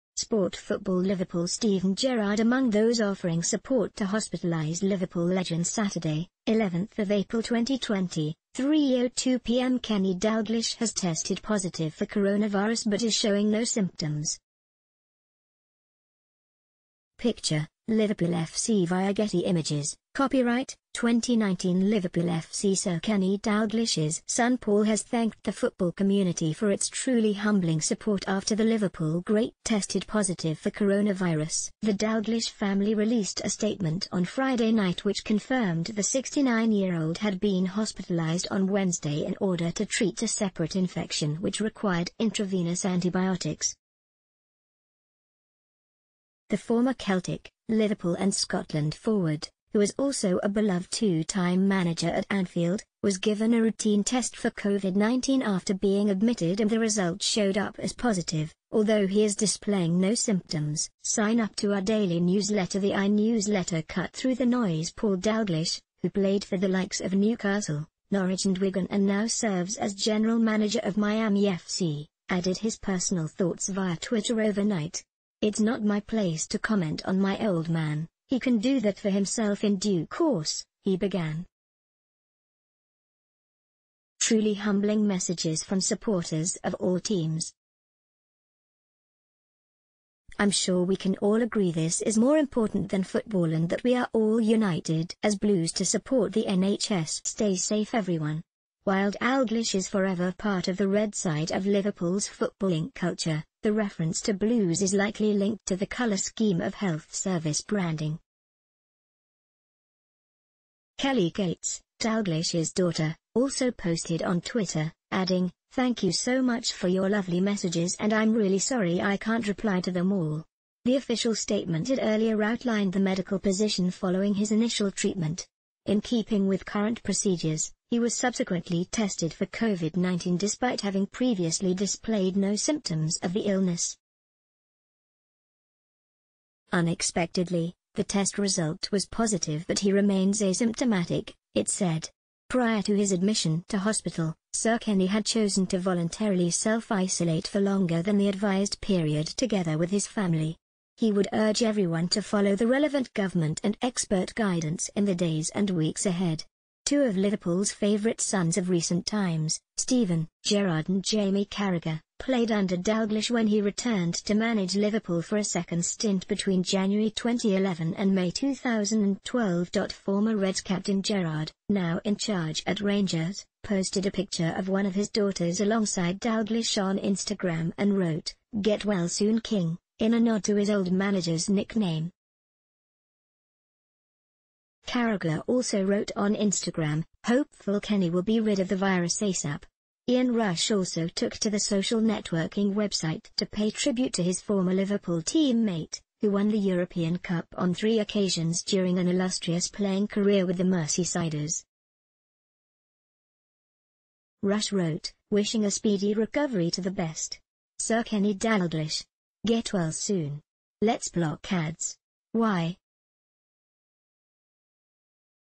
Kenny Dalglish’s family ‘humbled’ by public’s support after positive coronavirus test
SportFootballLiverpoolSteven Gerrard among those offering support to hospitalised Liverpool legendSaturday, 11th April 2020, 3:02 pmKenny Dalglish has tested positive for coronavirus but is showing no...